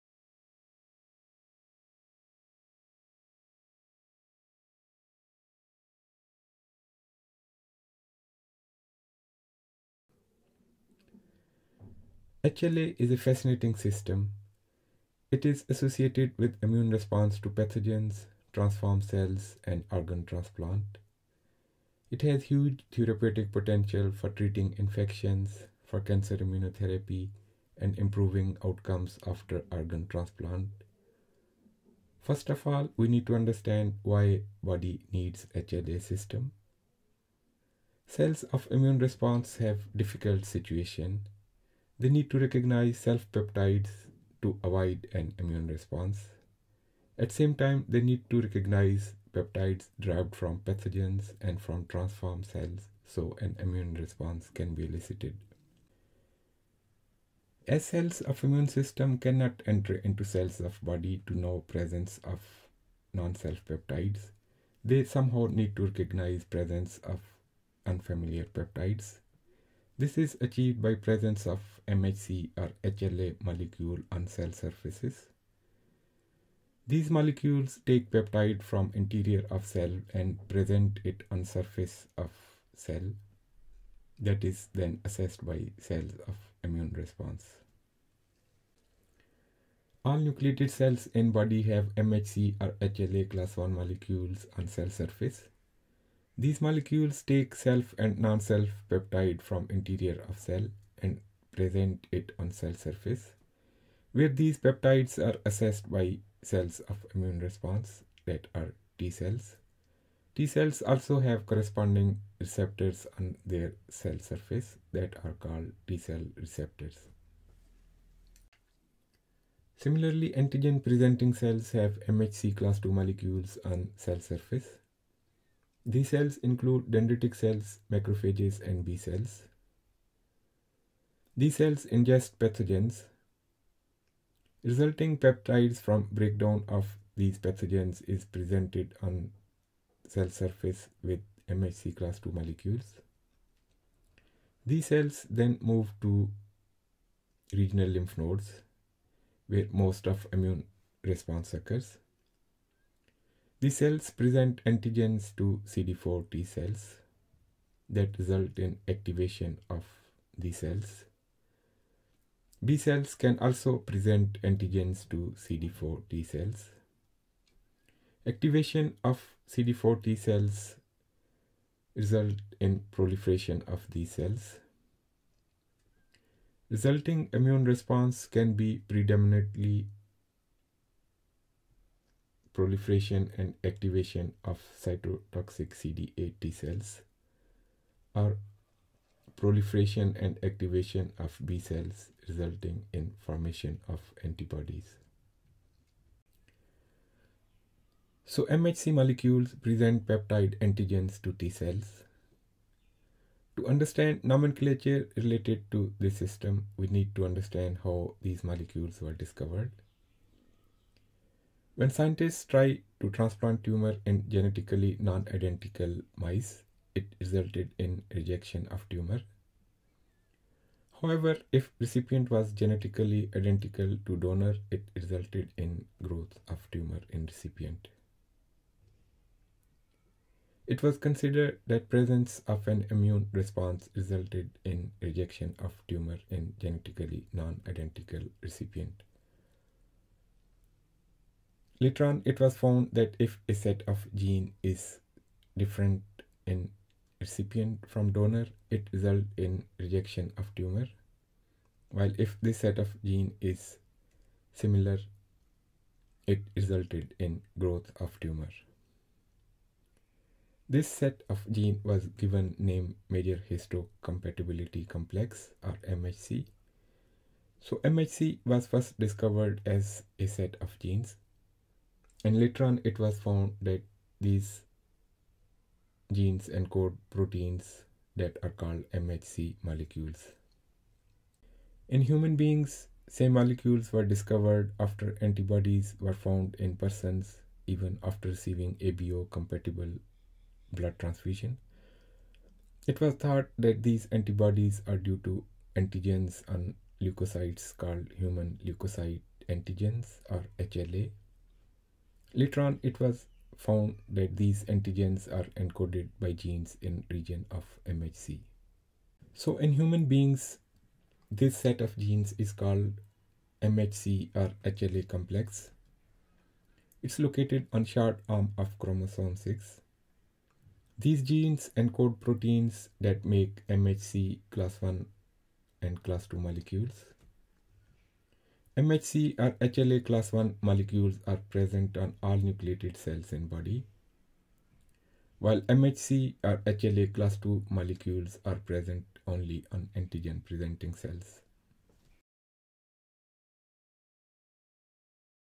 HLA Part 1: Major Histocompatibility Complex, HLA, Introduction, Discovery and Nomenclature
Last few decades have resulted in enormous development in this field. This system has huge potentials for treating infections, cancers and improving outcomes after organ transplant.
However, some aspects of this system have overlapping terms and lot of details. We will discuss all these aspects in this series.